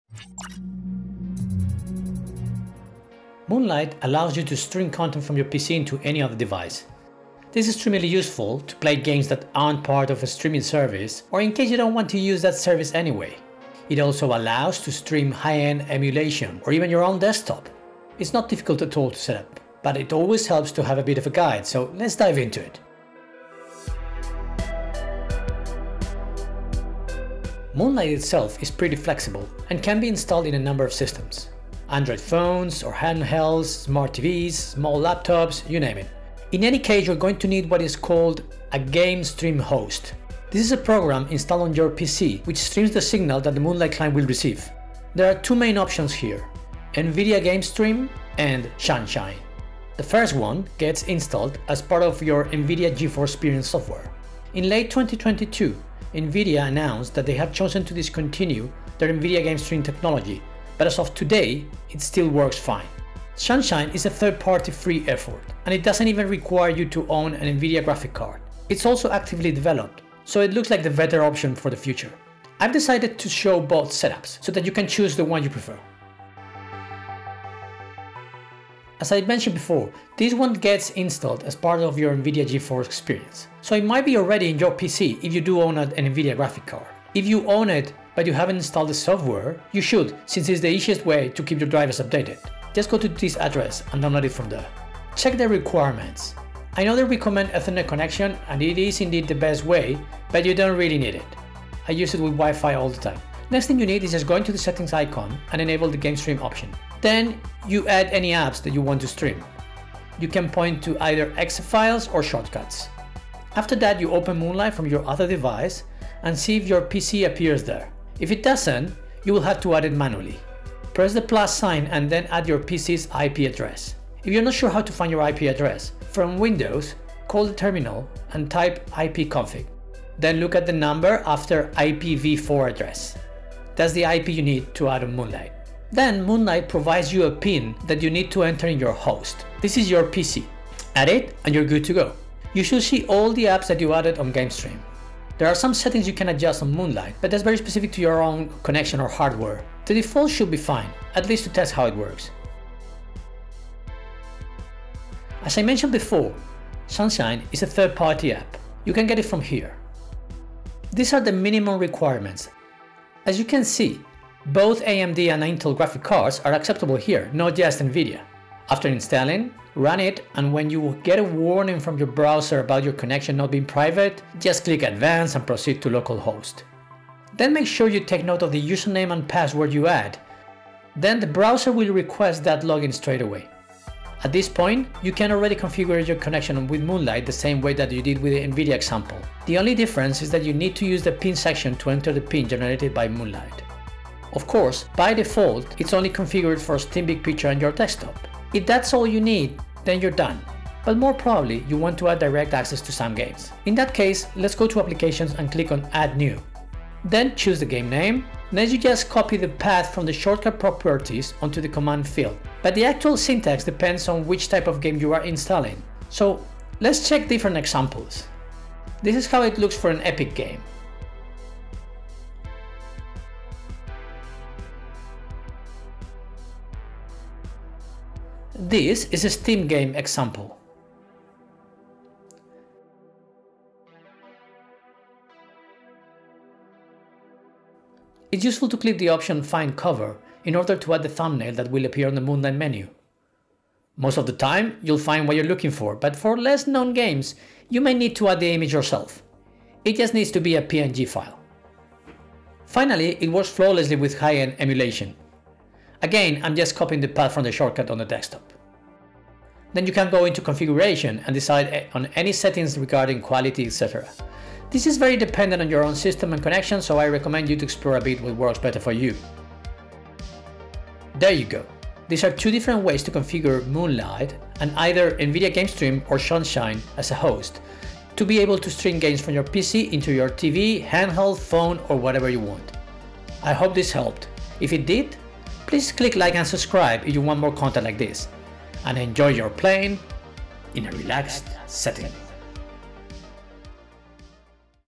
Moonlight Setup | How to stream with Nvidia Gamestream or Sunshine.
Learn how to stream with either Nvidia Gamestream or Sunshine into your Moonlight Client, to enjoy your PC games anywhere. I explain both hosts and go through both setups for your convenience.

EDIT: THE PART ABOUT NVIDIA GAMESTREAM ISN'T USEFUL ANYMORE, SINCE NVIDIA DROPPED THE SUPPORT FOR STREAMING ON THEIR NEW APP, AS I WARNED ON THIS VIDEO. THE PART ABOUT SUNSHINE IS STILL ABSOLUTELY RELEVANT AND USEFUL.

Note: Make sure to use one host OR the other. Not both at the same time, since they share the same port. I've found the easiest to install both, but enable or disable Gamestream from the Nvidia settings depending on which one I want to use.

Synthwave music by Karl Casey @ White Bat Audio

0:00 Intro
0:28 Moonlight
0:46 Two Hosts
1:18 Nvidia Gamestream
2:33 Sunshine
4:31 Outro